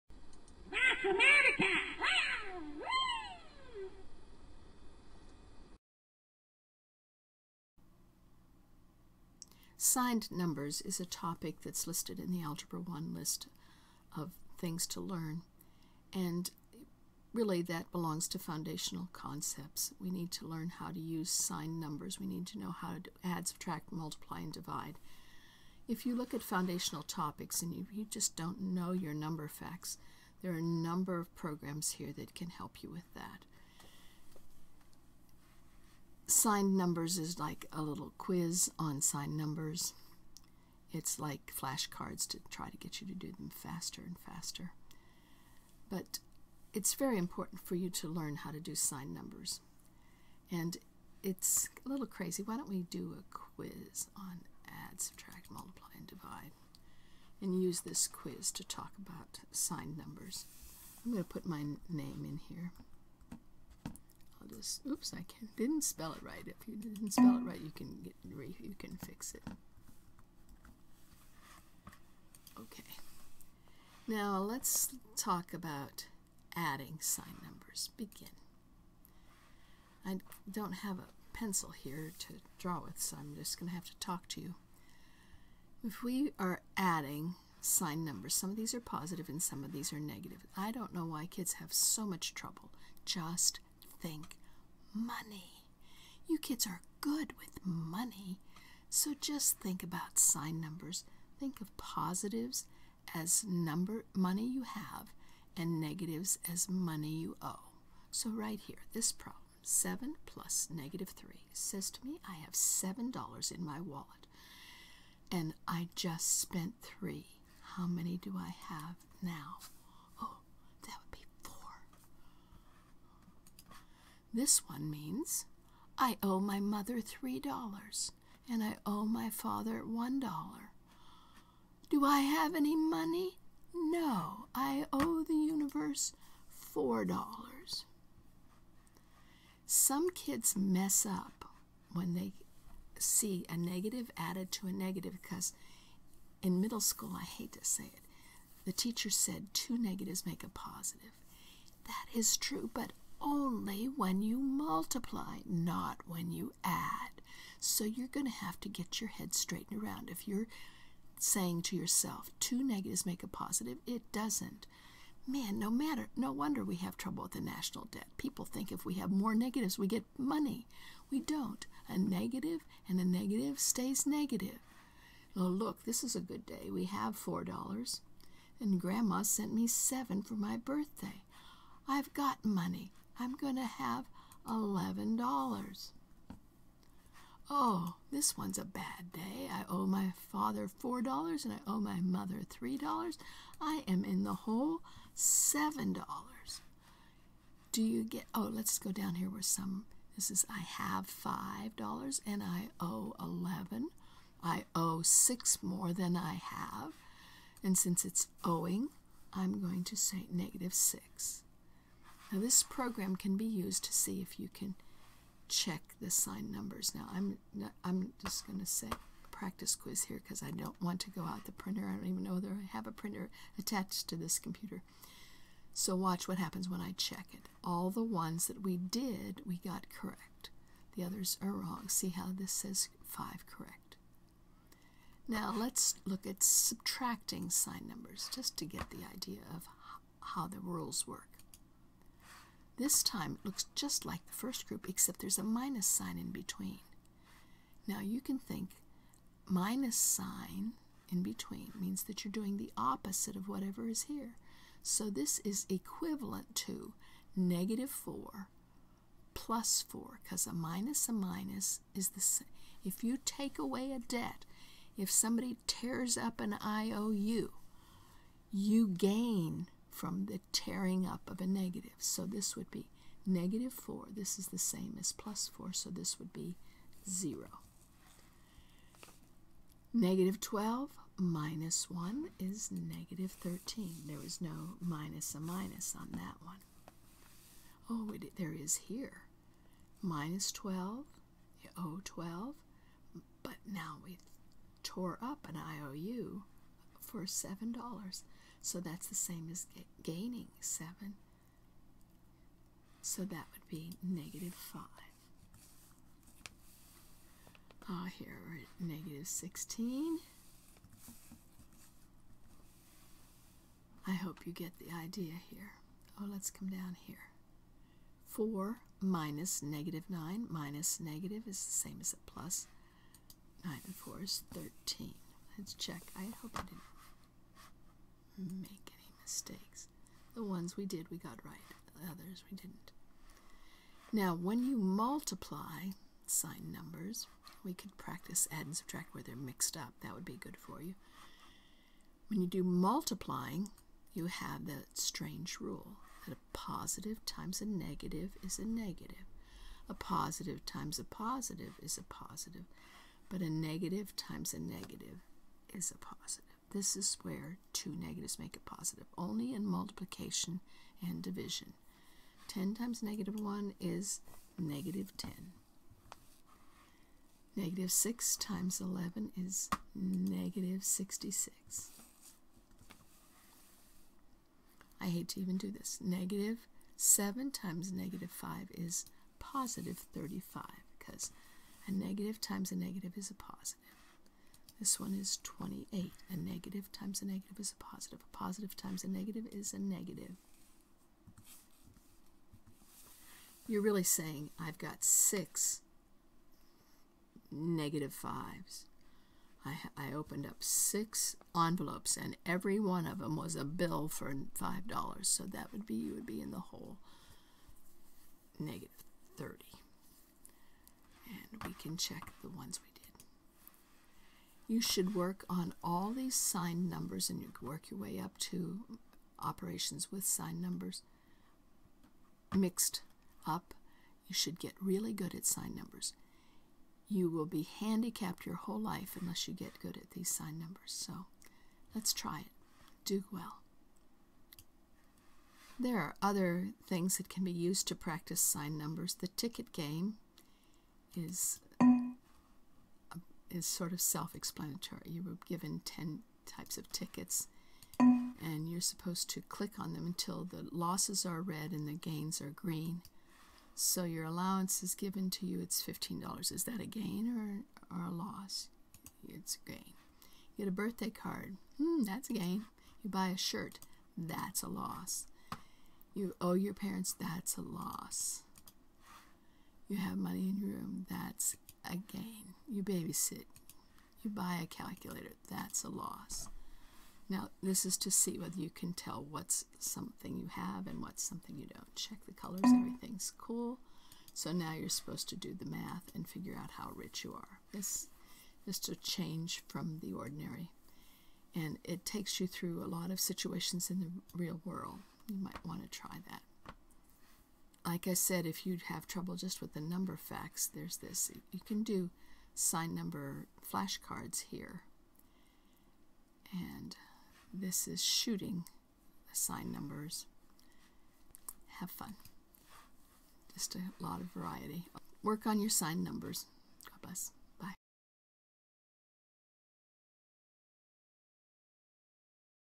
SignedNumberHelpQuiz TicketGame
Practice with signed numbers with the selfgrading quiz and the "Ticket Game" programs from Howe-Two Software